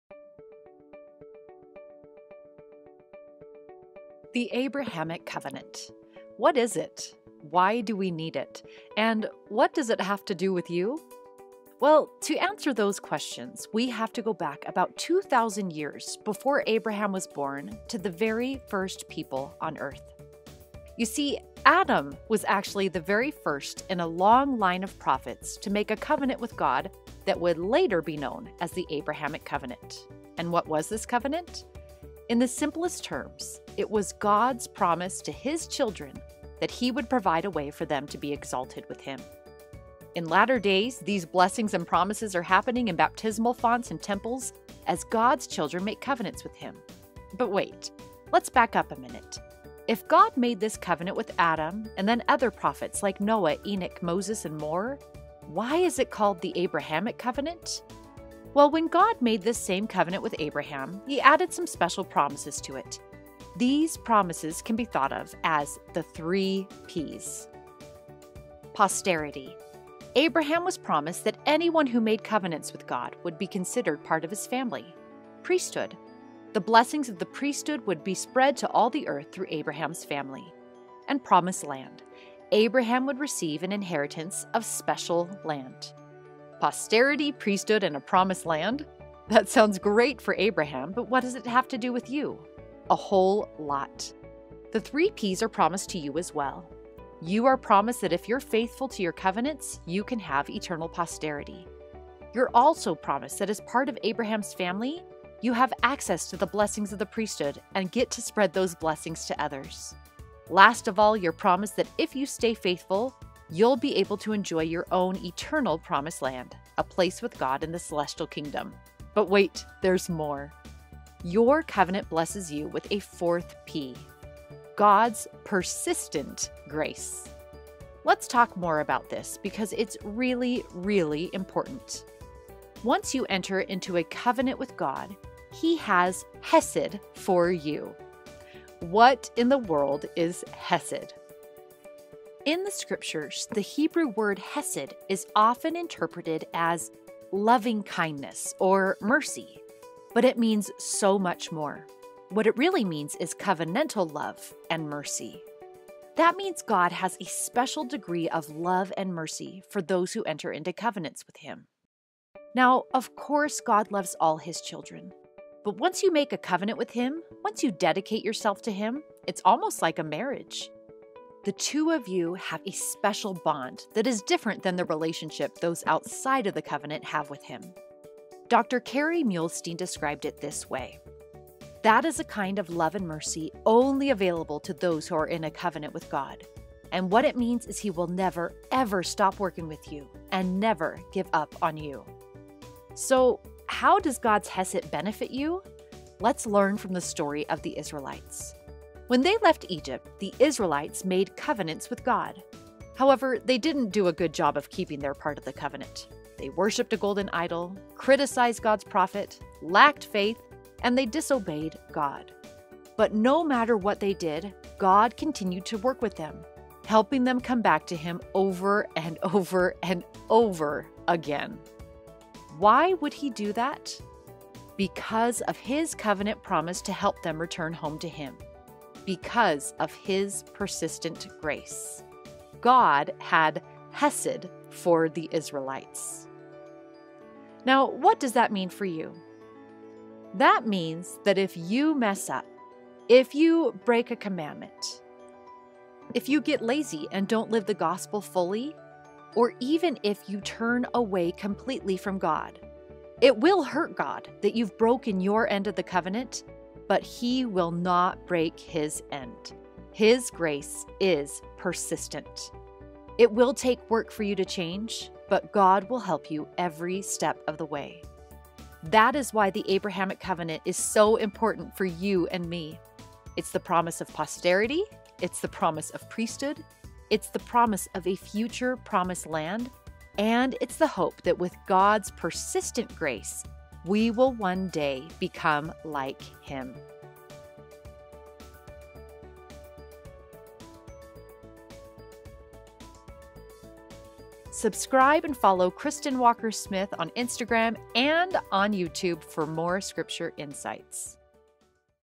The 4 P's of the Abrahamic Covenant (And Why It Matters For You!)
What is the Abrahamic Covenant?

And what does it have to do with you?

Watch this quick 5-minute video that is perfect for LDS families and Sunday classes that explains the modern-day application of the covenant God made with Abraham.

References:

Abraham 1:2-3 (Blessings of the Fathers from the beginning of time)
Abraham 2:9-11
Moses 5:7, 9
Moses 6:62, 68
D&C 84:34
D&C 132:30-33
Luke 3:8